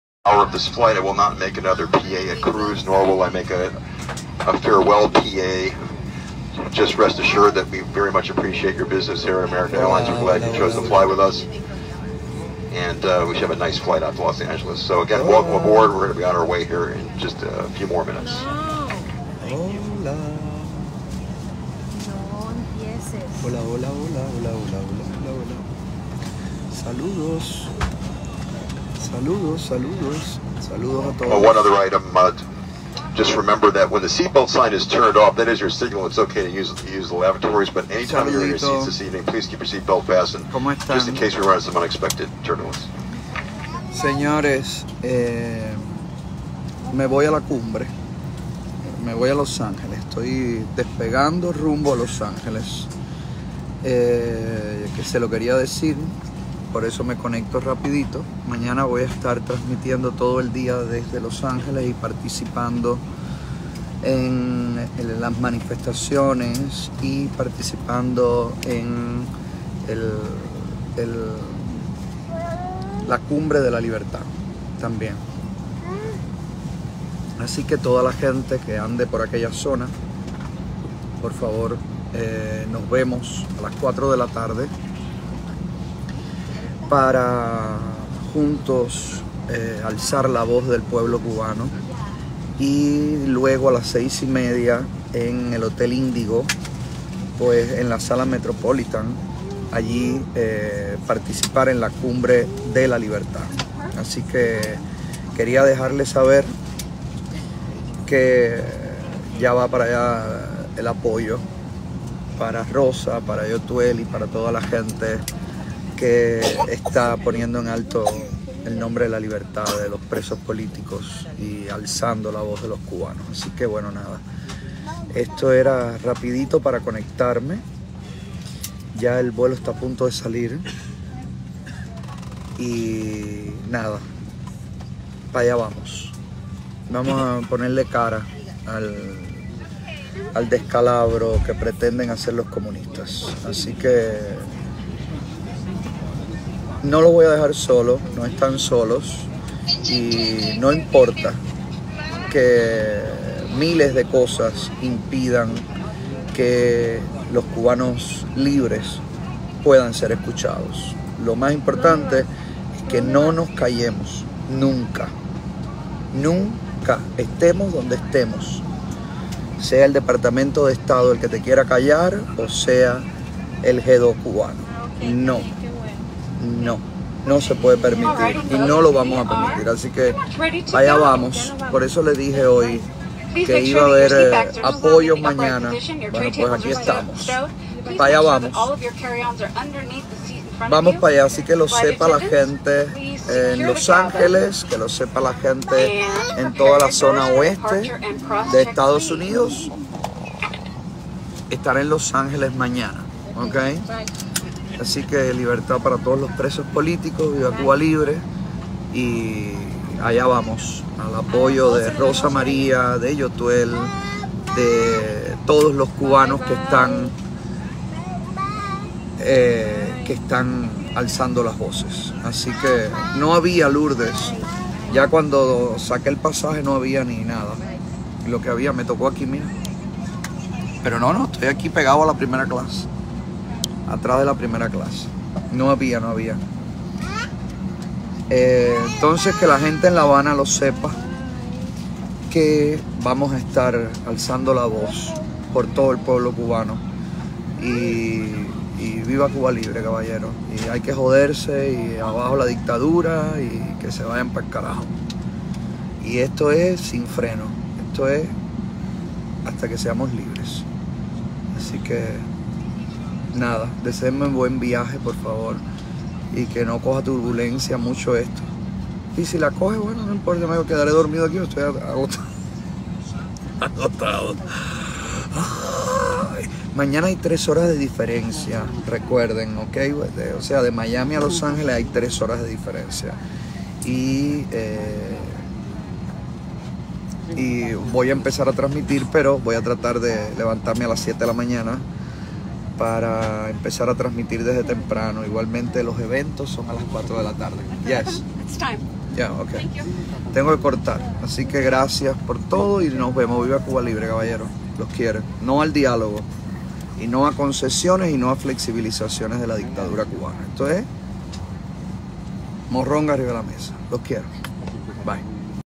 🔴SE FORMO!! Otaola se dirige a los Ángeles para participar en La cumbre de las Américas Sufre Diazca
👉 Para envios: Apartado de correos 122 Los Cristianos. 38650. Tenerife